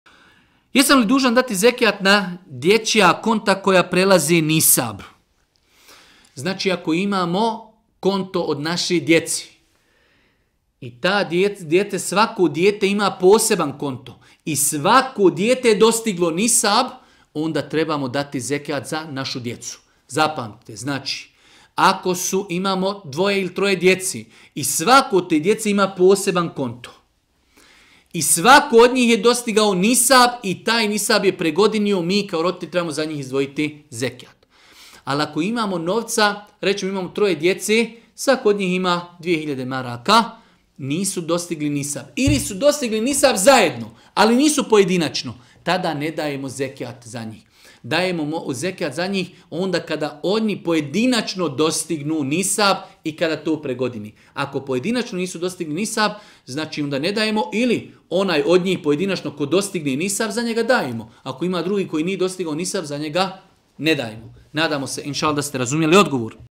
Jesam li dužan dati zekat na dječija konta ako prelaze nisab? - Elvedin Pezić
Ovaj video je isječak iz videa "Ramazanska pitanja i odgovori 10. dio" sa kanala Elvedina Pezića.